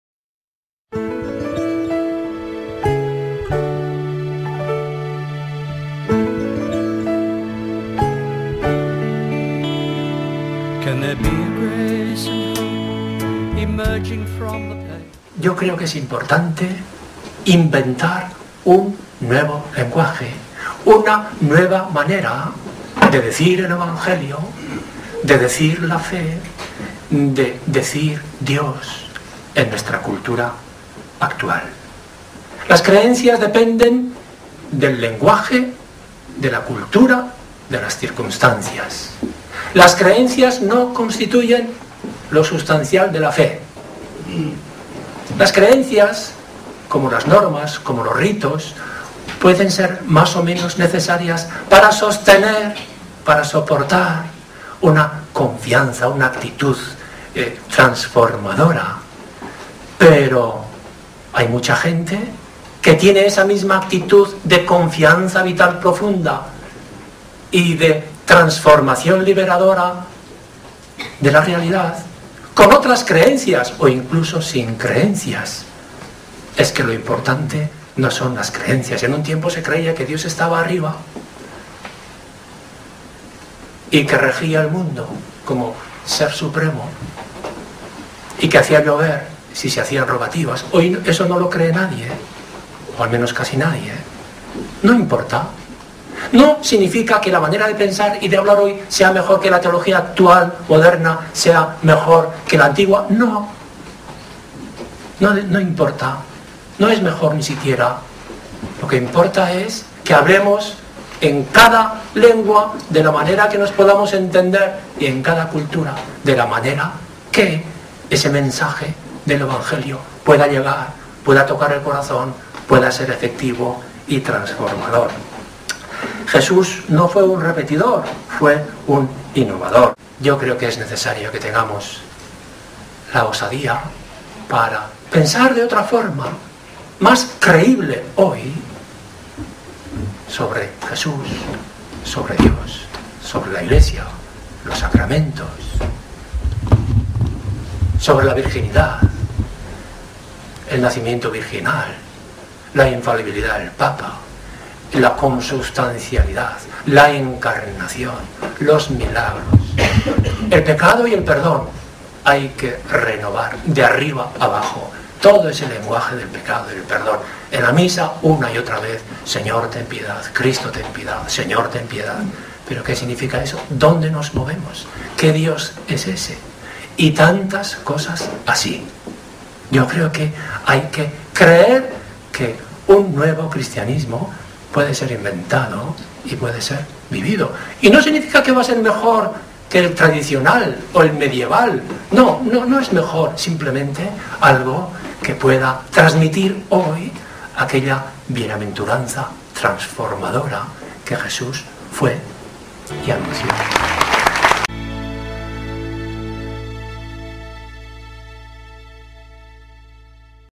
Thursdays of Theology in MYI. Jesus in the 21st century. Joxe Arregi (9/9)
8. Inventing a new language.

It's important, I believe, to invent a new language. Invent a new way to tell the gospel, to talk about faith and God in our culture today.

I think we have to believe that a new Christianity can be invented and can be lived. And it does not mean that it will be better than the traditional, or medieval Christianity. No, it will not be better. It will just be a language of today that can communicate the transforming happiness that Jesus lived and preached.